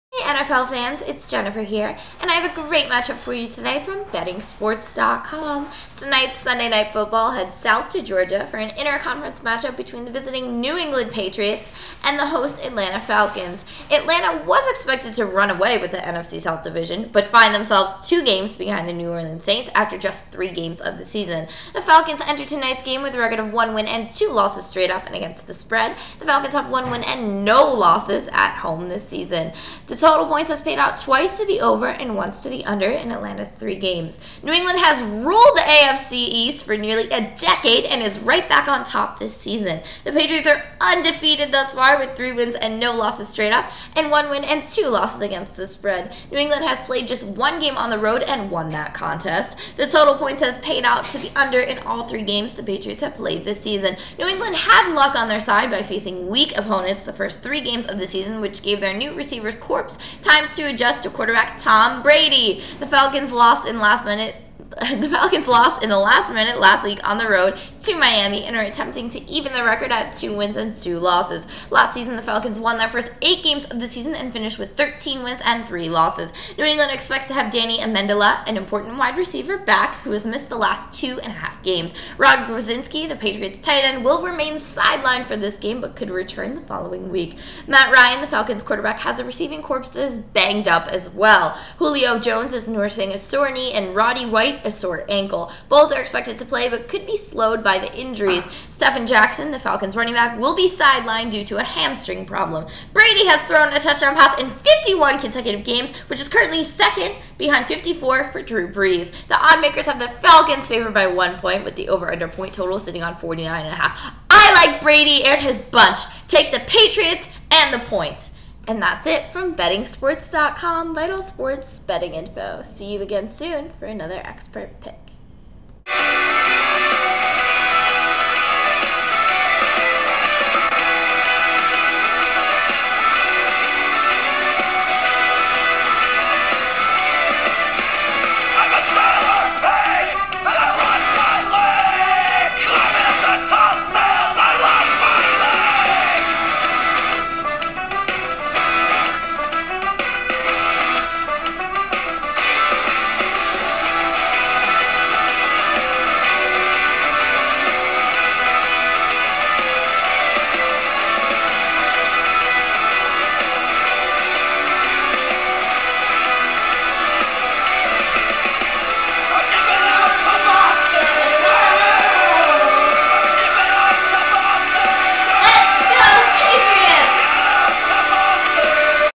Free Pick 09/29/13 -- Patriots vs Falcons Game Line
Tonight the New England Patriots head south to take on the Atlanta Falcons in a matchup of two teams considered to be in the postseason. Atlanta has started slow at 1-2, while New England is undefeated through the first three weeks of the seasons. Be sure and get all the information you need on this game, and more